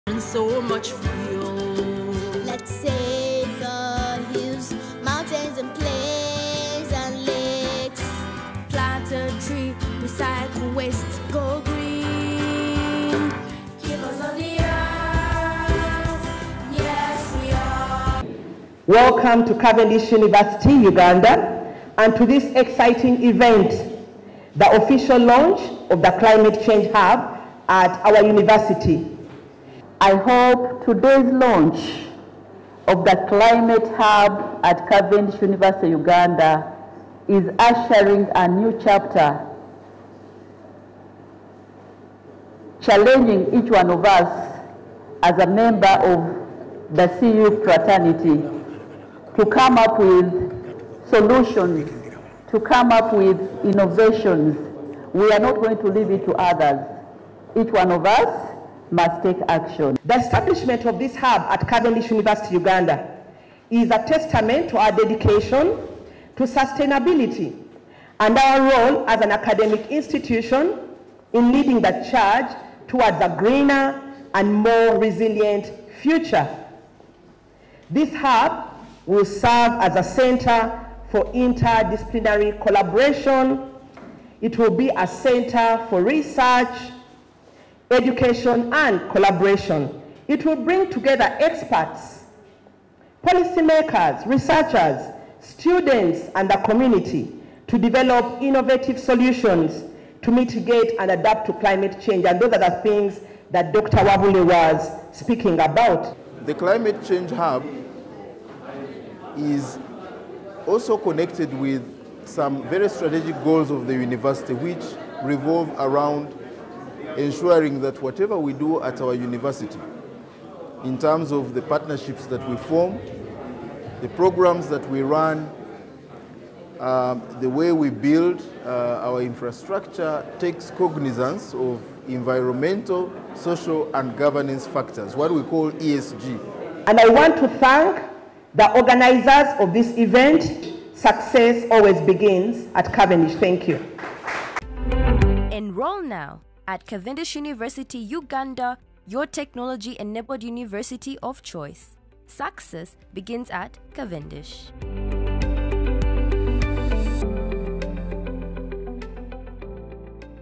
Highlights from the Launch of the CUU Climate Change Hub
This Hub is a testament to our unwavering dedication to sustainability and our pivotal role as an academic institution in spearheading the movement towards a greener and more resilient future.

The hub will serve as a focal point for interdisciplinary collaboration, research, and transformative education. By uniting experts, policymakers, researchers, students, and the community, we aim to cultivate innovative solutions to combat and adapt to the challenges posed by climate change.